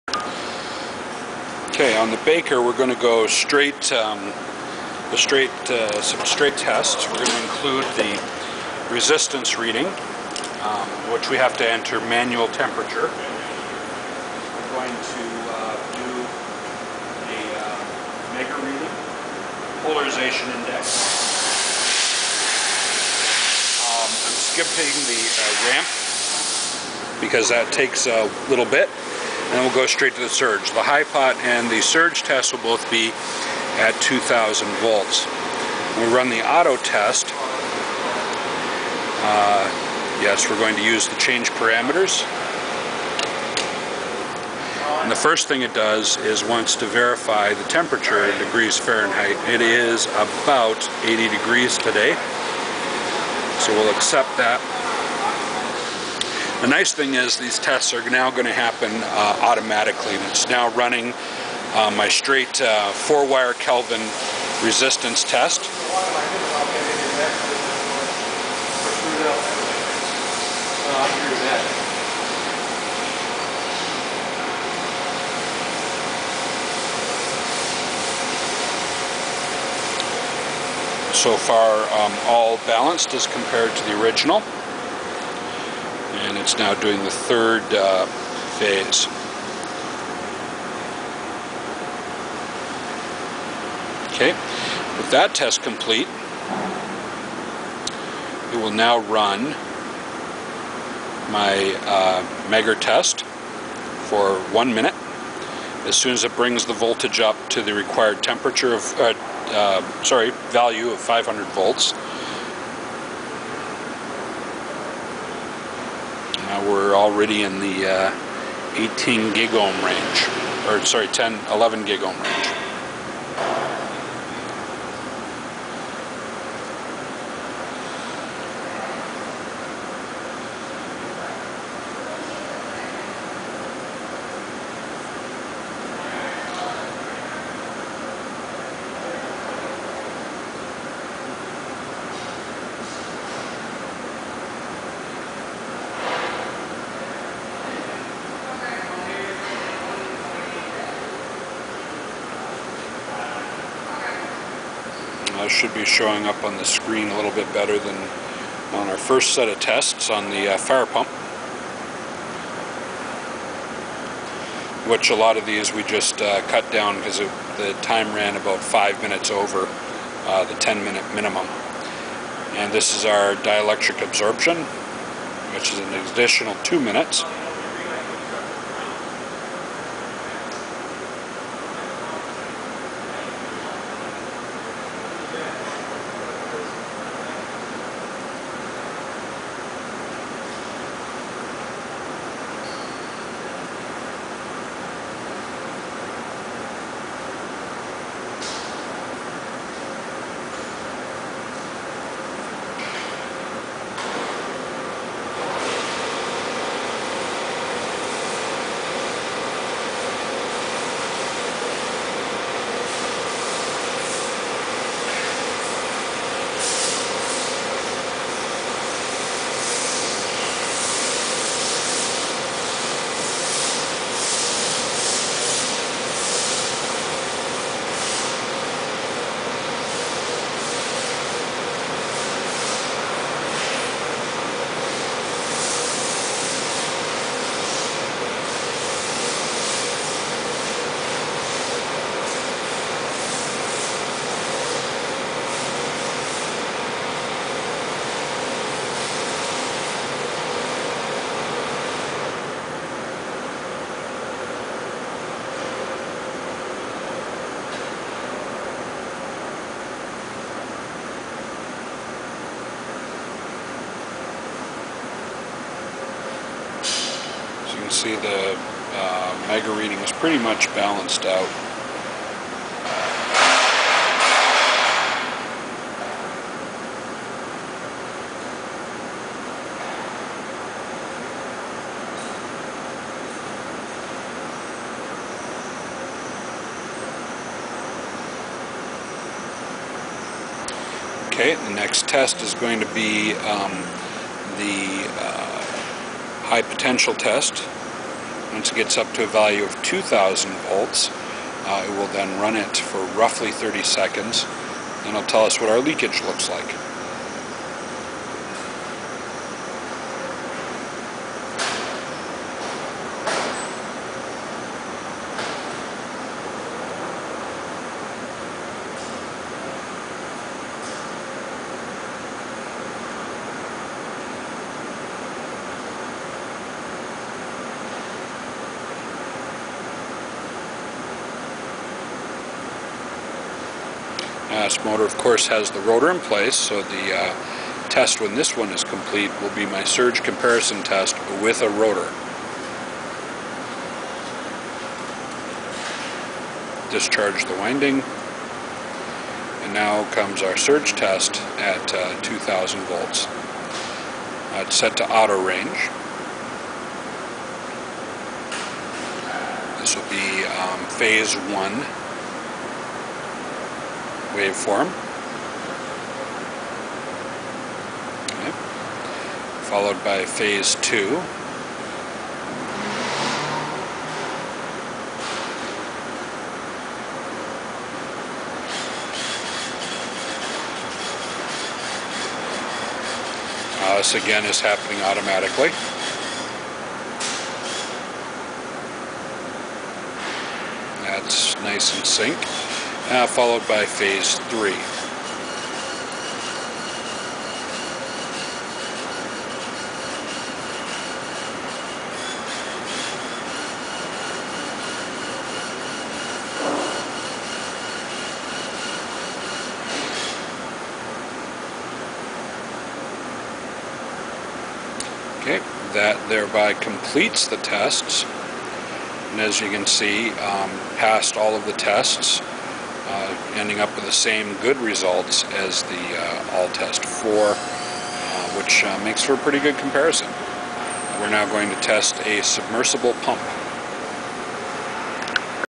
Compare ALL-TEST IV Pro 2000 to SKF Baker AWA II Part 4
Evaluation of a Lafert motor.  About 20 hp.  Second of two parts.  Skipped the nameplate entry on the AWA